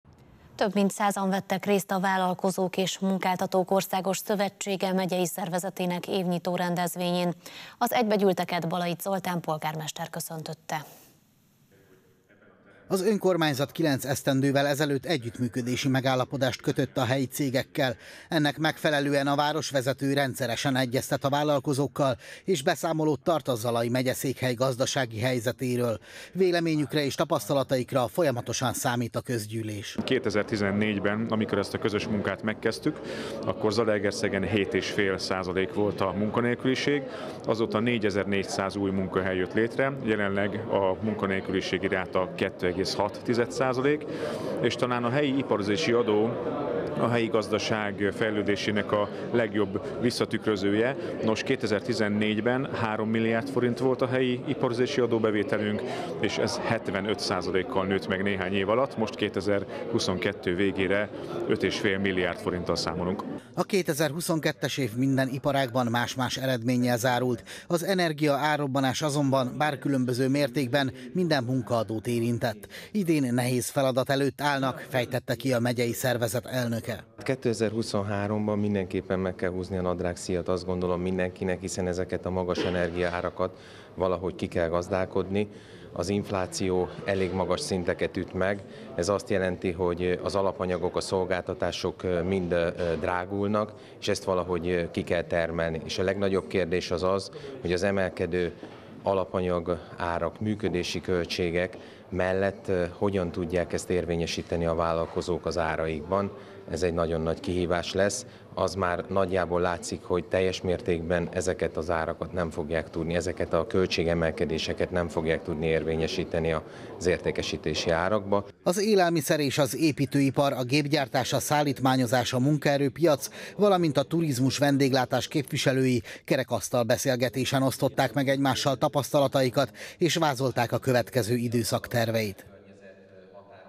Évnyitó – ZTV Híradó 2023-01-23
Több mint százan vettek részt a Vállalkozók és Munkáltatók Országos Szövetsége megyei szervezetének évnyitó rendezvényén. Az egybegyűlteket Balaicz Zoltán polgármester köszöntötte.